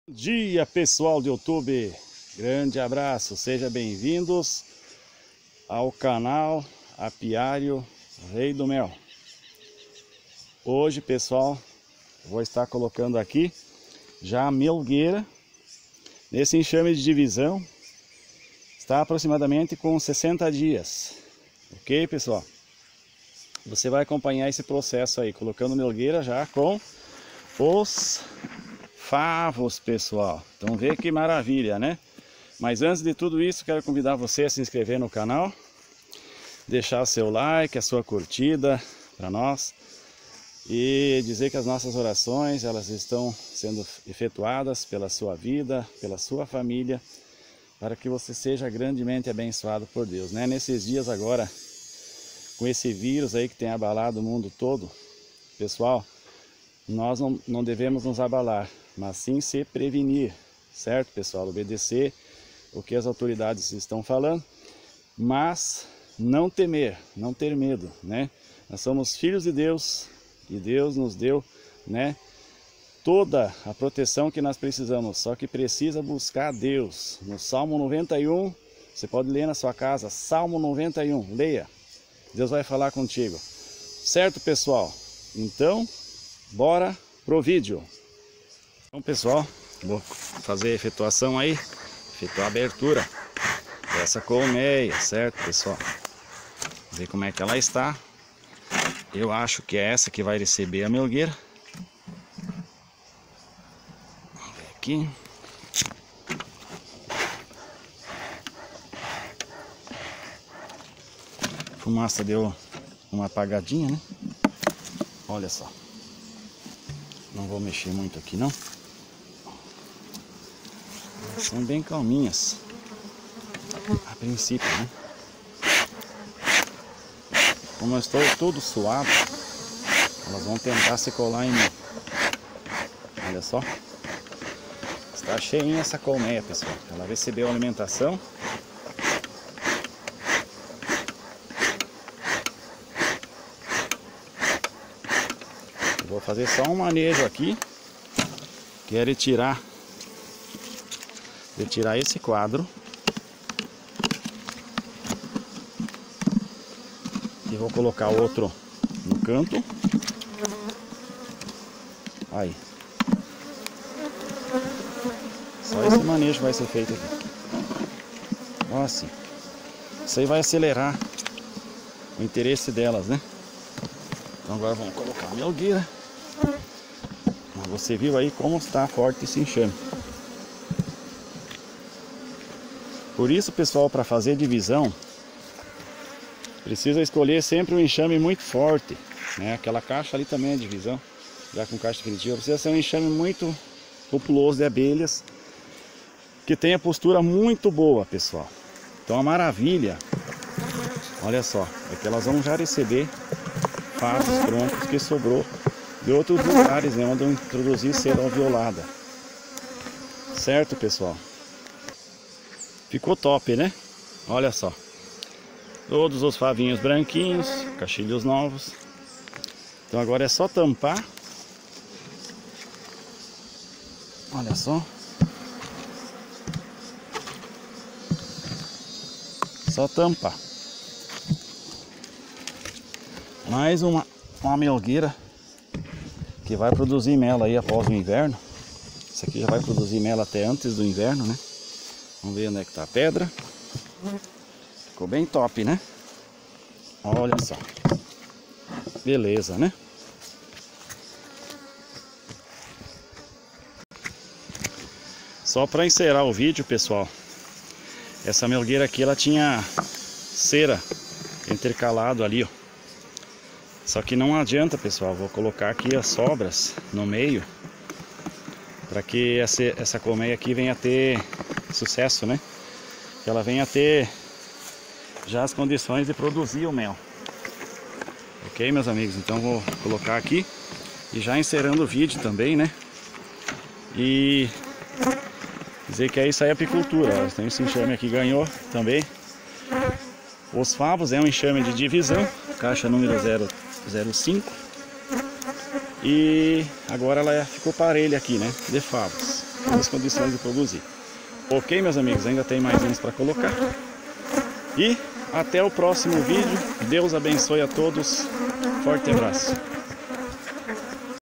COLOCAÇAO DA MELGUEIRA
Esse enxame é de divisao. Tem uns 60 dias e recebeu melgueiras com os favos. Também outros enxames receberam favos que sobraram da retirada do mel